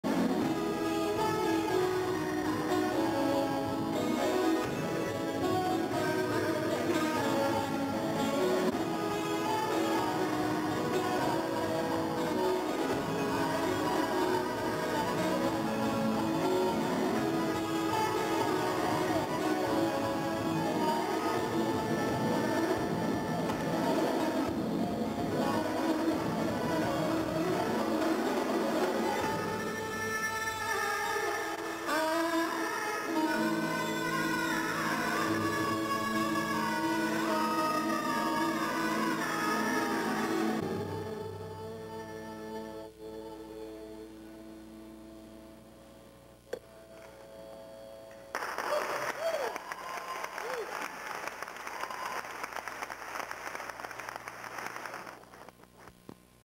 Sandip Burman with the Arizona State Orchestra
Arizona State University commissioned this piece by Sandip Burman.
The auditorium was filled to capacity so the audience overflow was allowed to sit on choir risers behind the musicians.

Gary Hill: Conductor
Sandip Burman: Tabla
Dave Pietro: Saxophone
Sima Burman: Voice & Harmonium
Paul Bollenback: Guitar
also featuring the ASU Orchestra